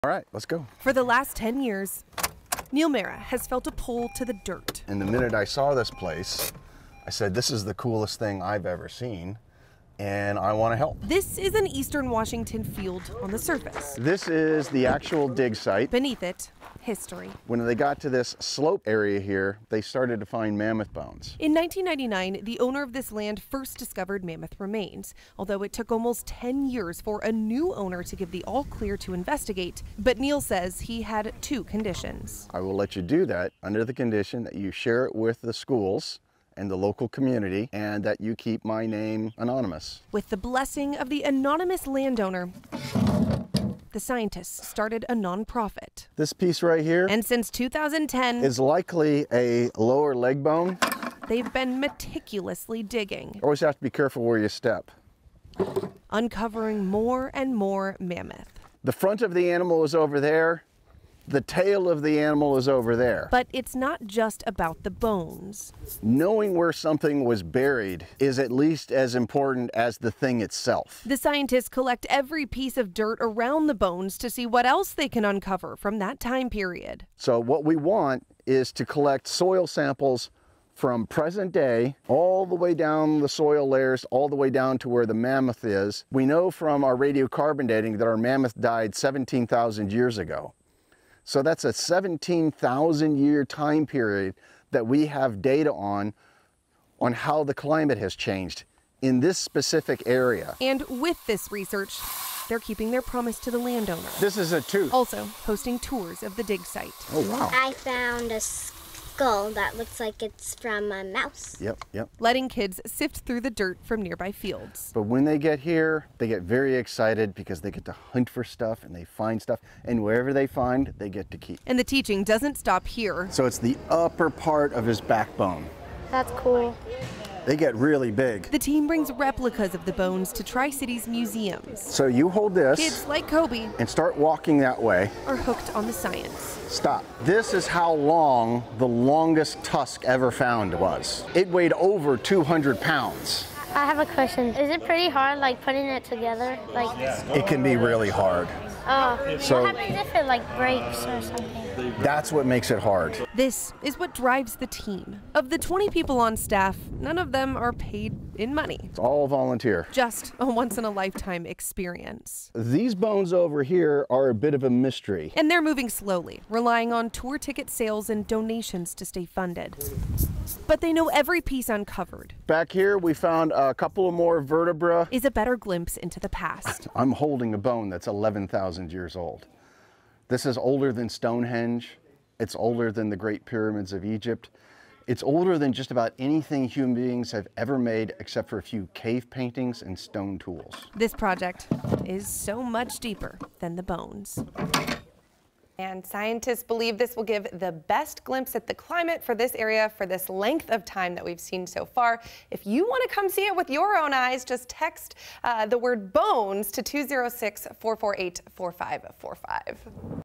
Tri-Cities volunteers slowly digging up mammoth remains
Their goal is to uncover not just the bones but everything around the remains and to educate the public.